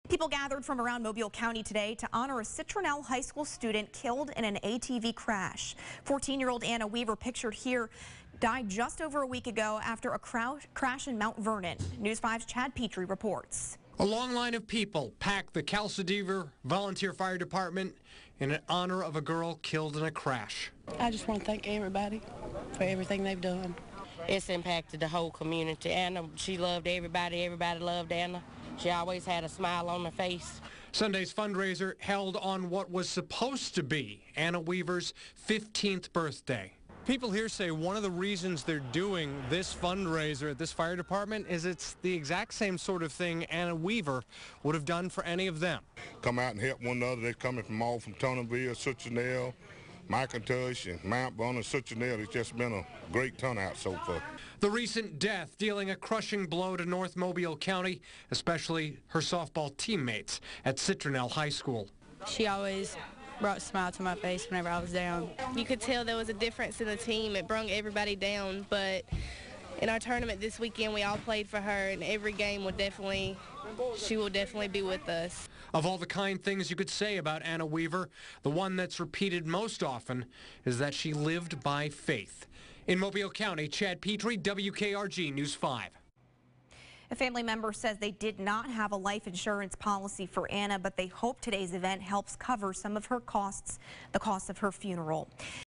Benefit Sunday for Citronelle teenager killed in ATV crash
People are gathering to remember the life of Anna Weaver on what would have been her 15th birthday.